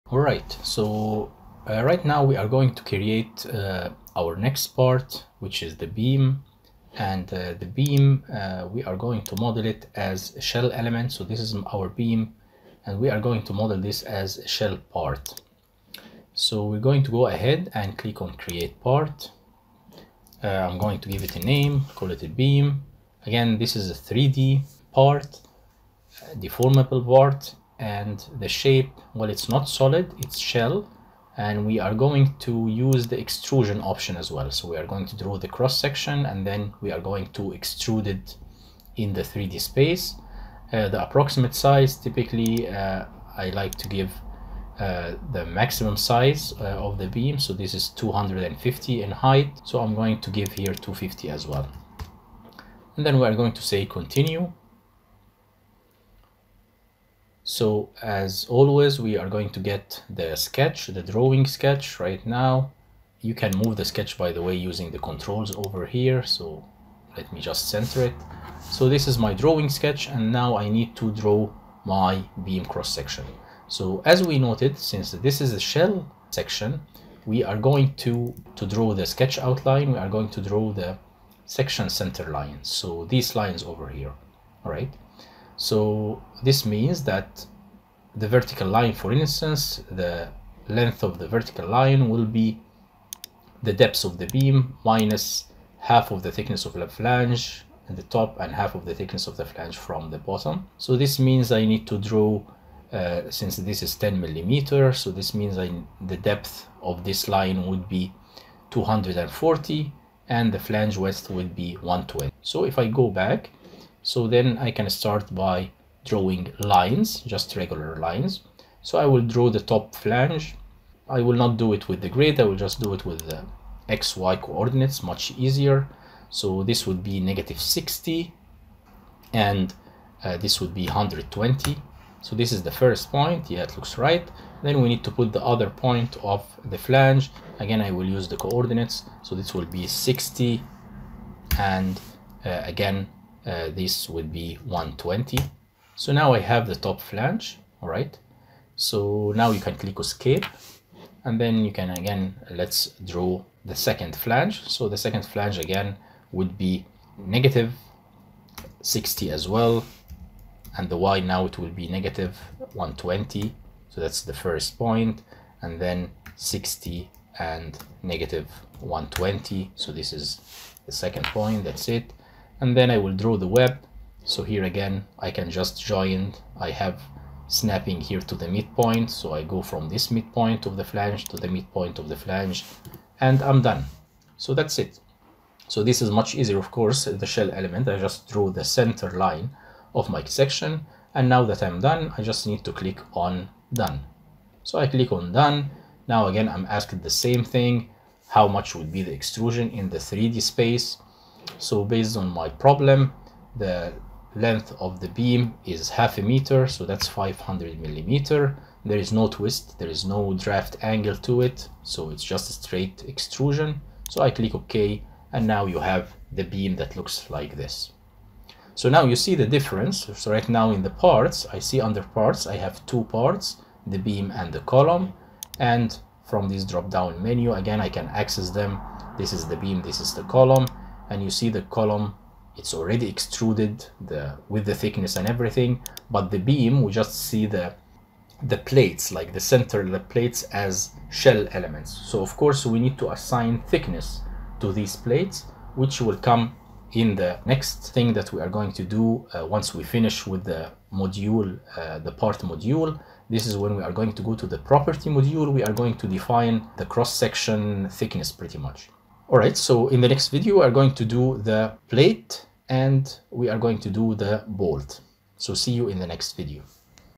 #04 ABAQUS Tutorial: Creating a shell part - I-shaped beam
How to create a deformable shell part for an I-shaped beam in ABAQUS.